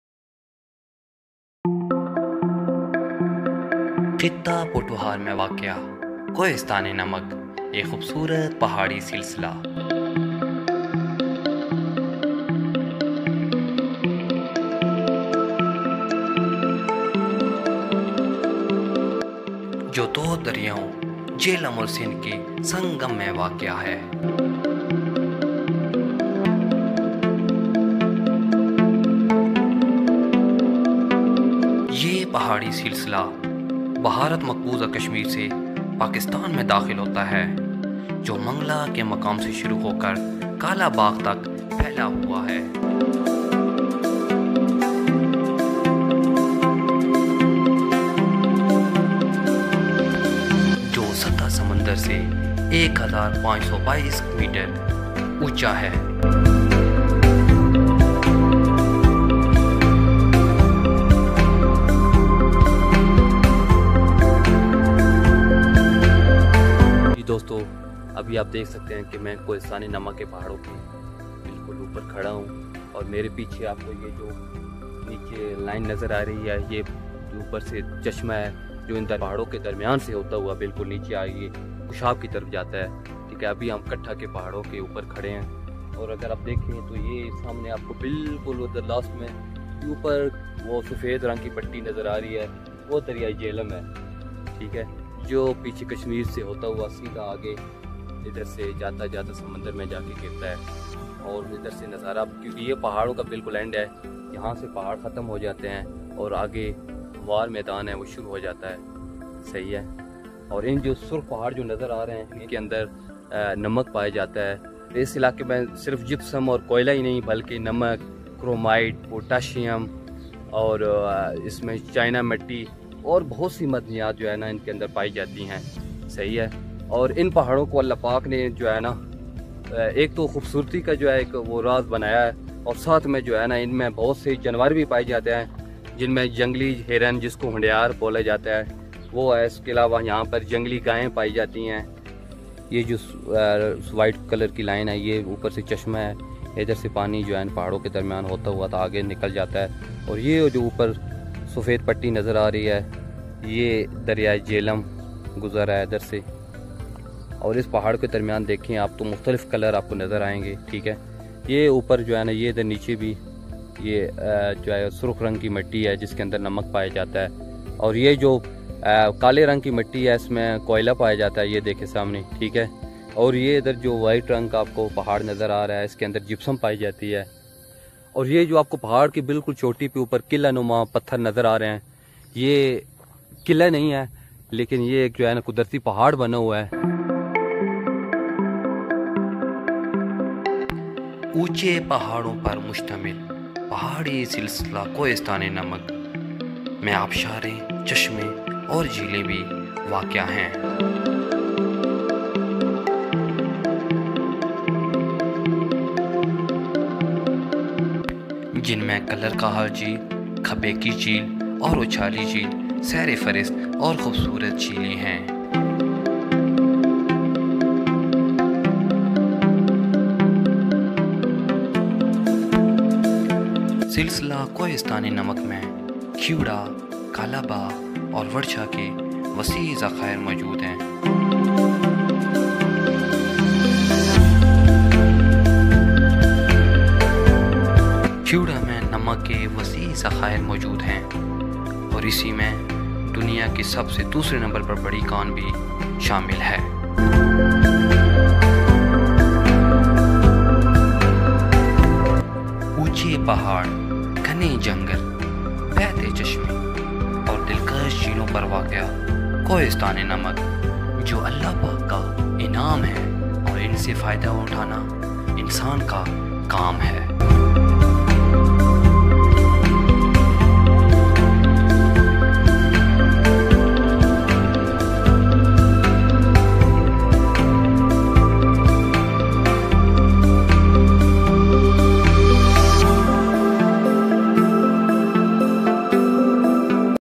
Kohistan-e-Namak#phothohar#Documentary
Kohistan-e-Namak is beautiful salt range between two rivers jhelum and sindh .